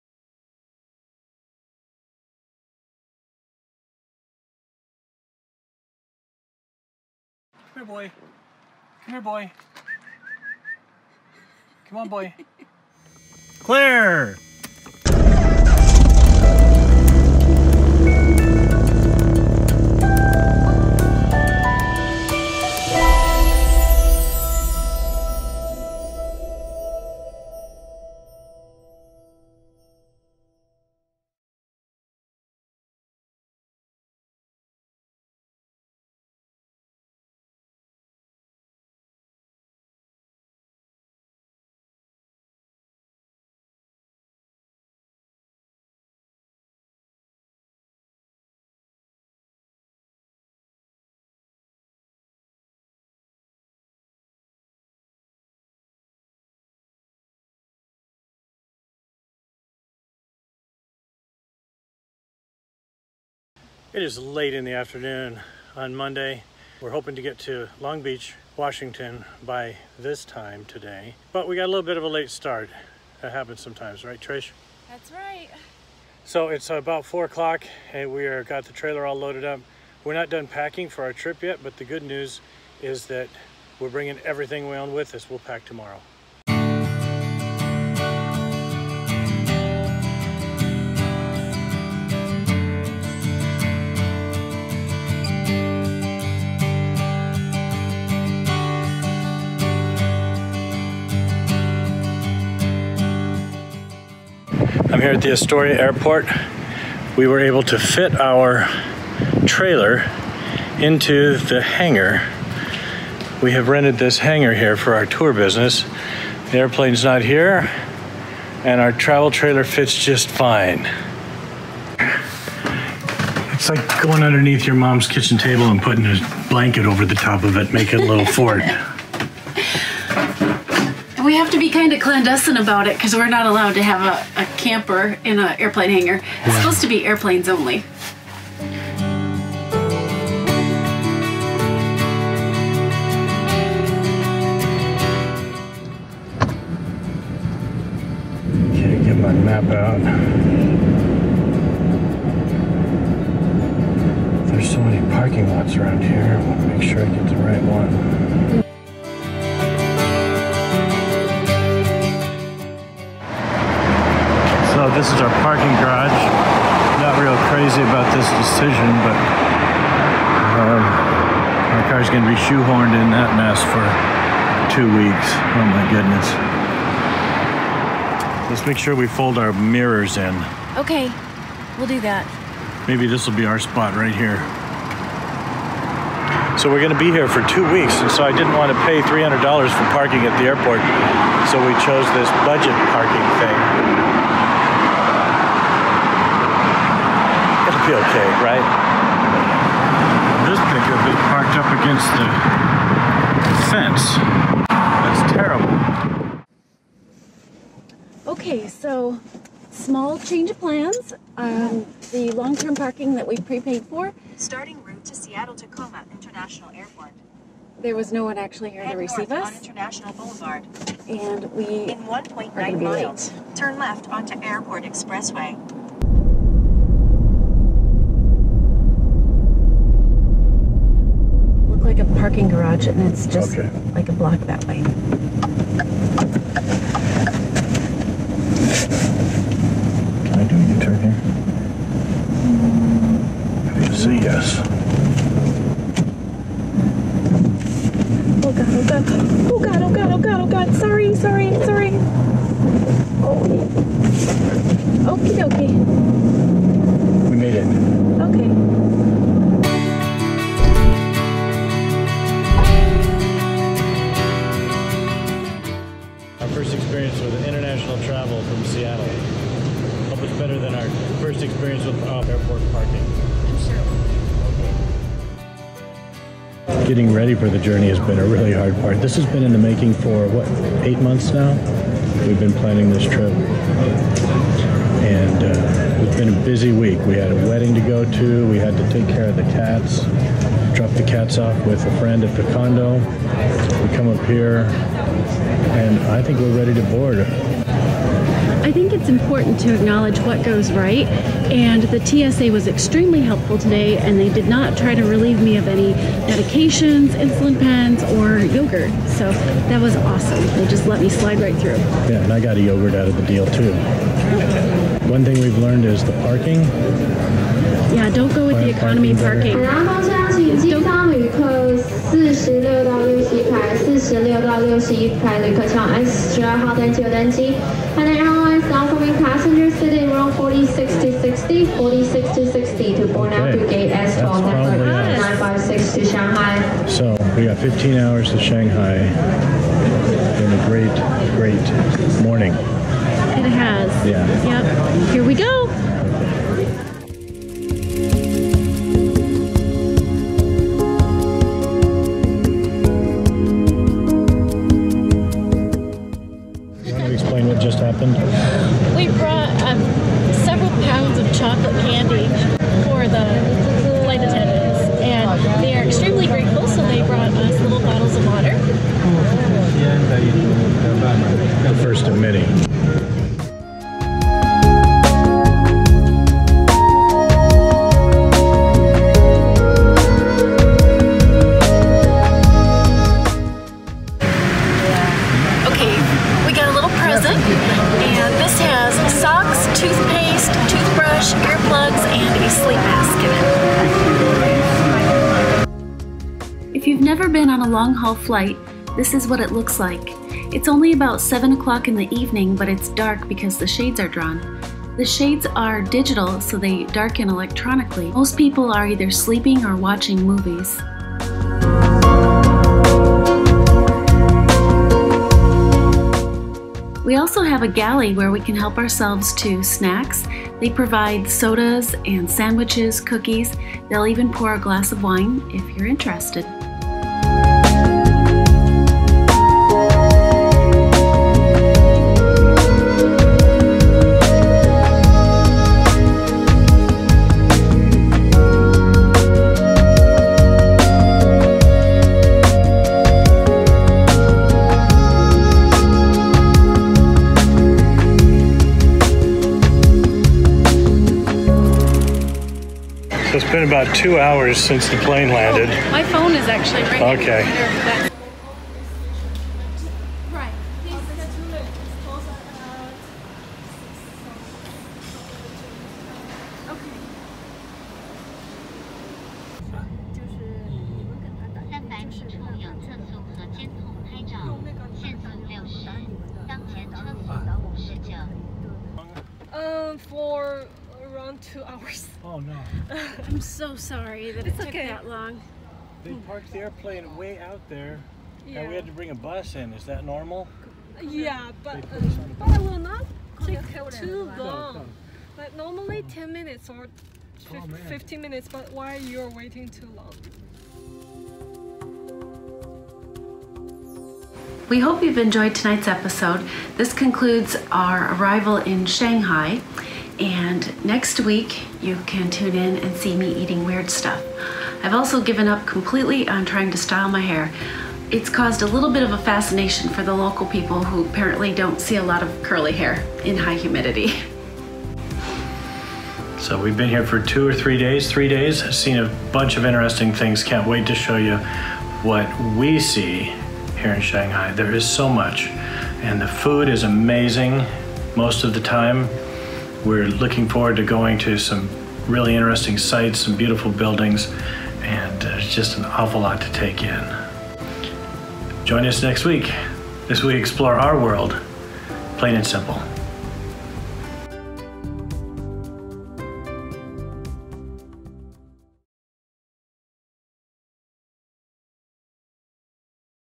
Strangers in a Strange Land
Trish and TF prepare to travel from Seattle, WA to Shanghai, China. As usual, not everything goes according to plan...